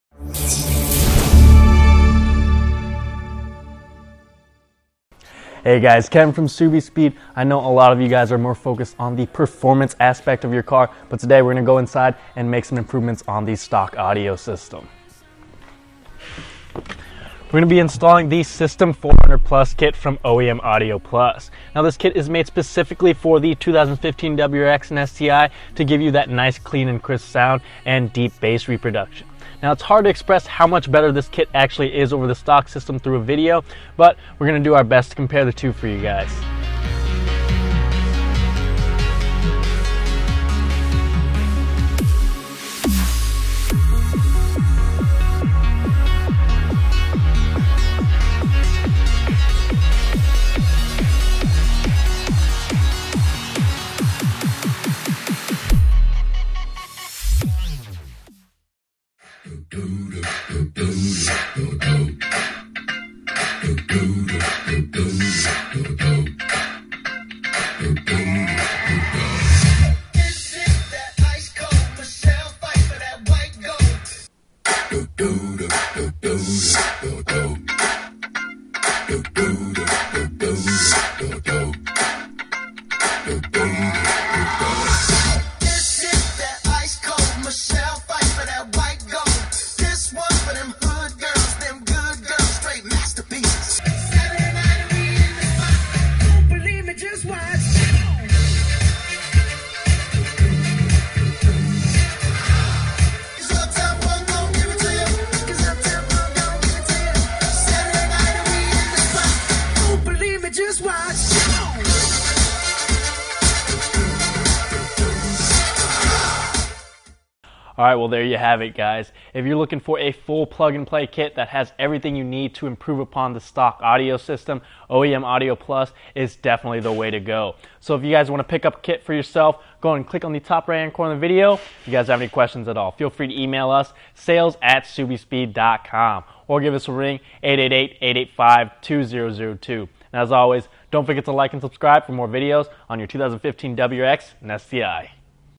Subispeed - 2015 WRX & STI OEM Audio+ System 400+
Let's be honest, the stock audio system on the new 2015 WRX & STI isn't spectacular. That's exactly why OEM Audio+ came out with a a full plug'n'play upgrade that drastically improves the quality of your stereo system! Check it out for yourself!